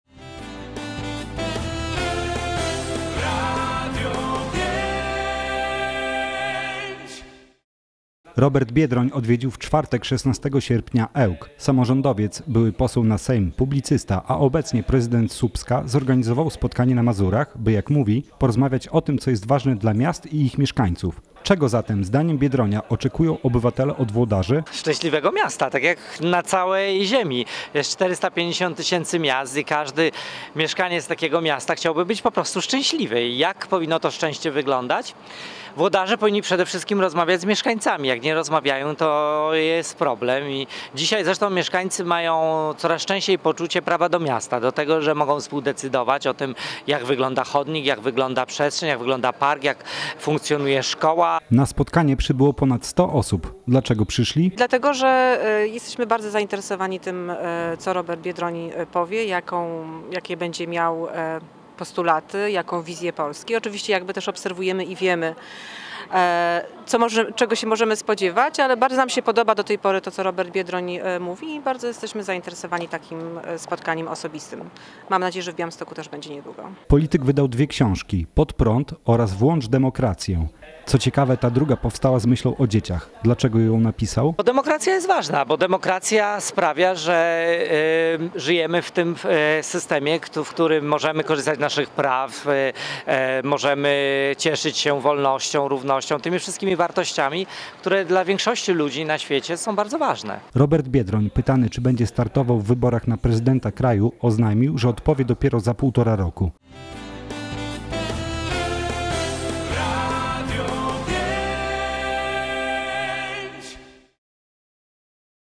Robert Biedroń odwiedził Ełk
Robert Biedroń odwiedził w czwartek (16.08) Ełk. Samorządowiec, były poseł na sejm, publicysta, a obecnie prezydent Słupska zorganizował spotkanie na Mazurach, by jak mówi - porozmawiać o tym, co jest ważne dla miast i ich mieszkańców. Na spotkanie przybyło ponad sto osób. Polityk wydał dwie książki: „Pod prąd” oraz „Włącz DEMOkrację”. Co ciekawe, ta druga powstała z myślą o dzieciach. Robert Biedroń pytany, czy będzie startował w wyborach na prezydenta kraju, oznajmił, że odpowie dopiero za półtora roku.